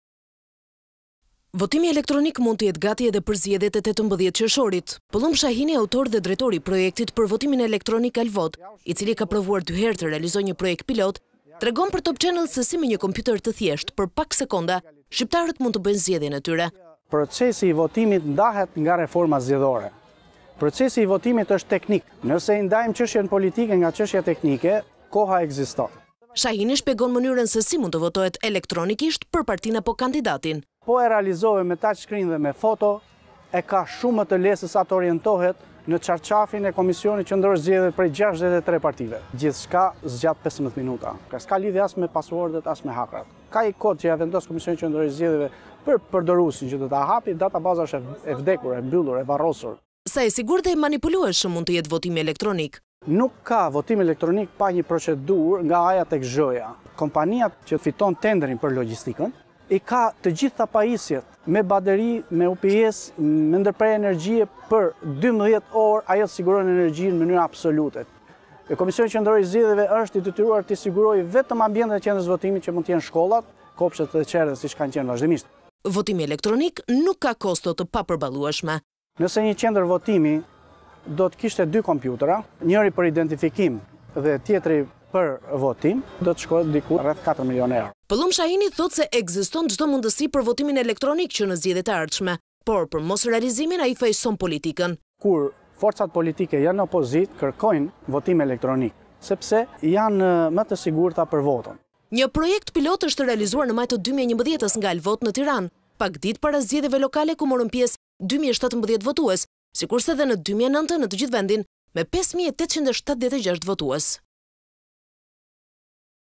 Shahini: Votimi elektronik, i mundur. Kostoja, 3-4 mln euro - Top Channel Albania - News - Lajme
Votimi elektronik mund të jetë gati edhe për zgjedhjet e 18 qershorit. Pëllumb Shahini, autor dhe drejtor i projektit për votimin elektronik “ALVOT”, i cili ka provuar dy herë të realizojë një projekt pilot, tregon për Top Channel sesi me një kompjuter të thjeshtë, për pak sekonda, shqiptarët mund të bëjnë zgjedhjen e tyre.